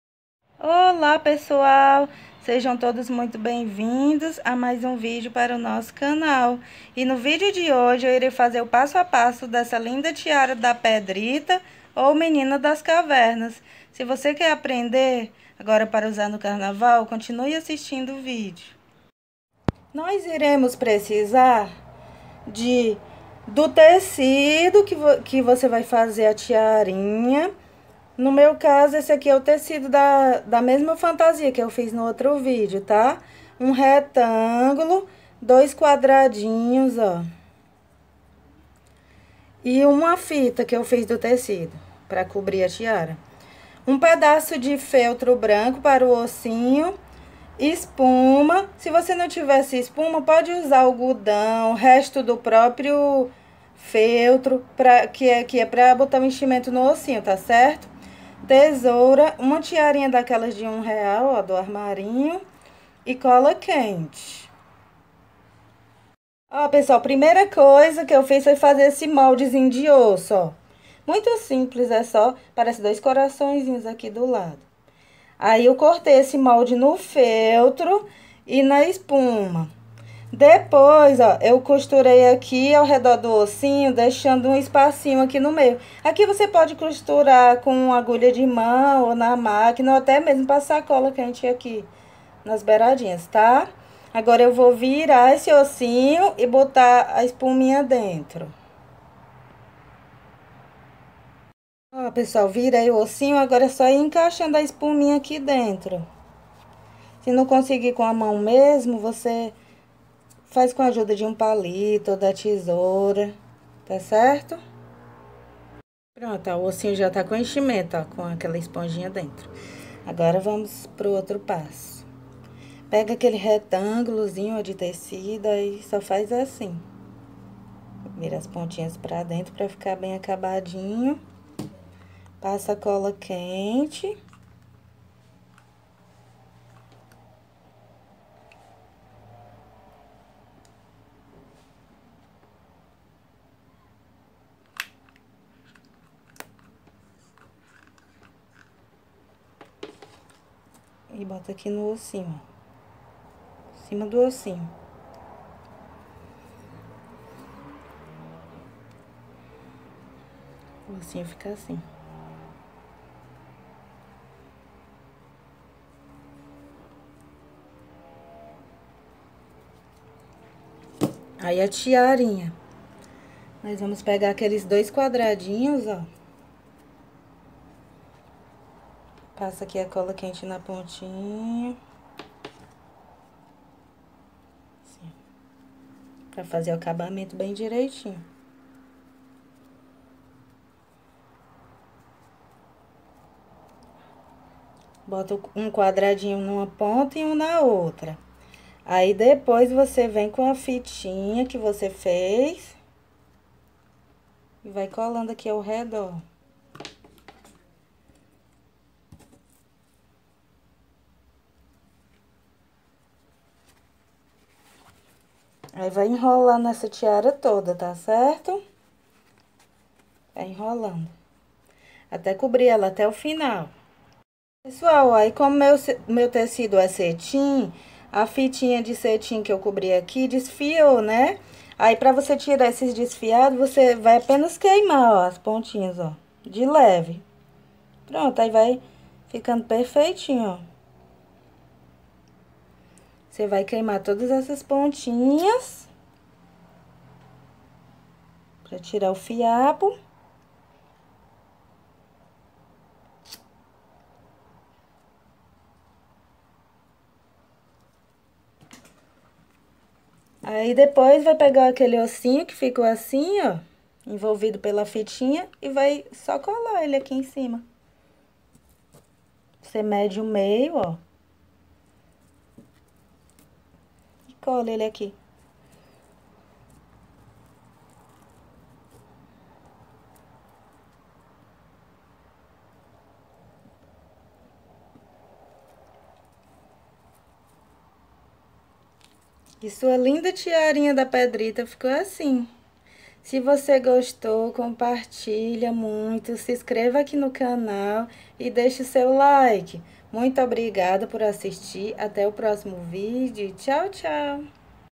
COMO FAZER a tiara da Pedrita
DIY tiara da Pedrita
Olá pessoal o vídeo de hoje é um passo a passo de como fazer essa linda tiara da Pedrita ou menina das cavernas muito fácil de fazer!carnaval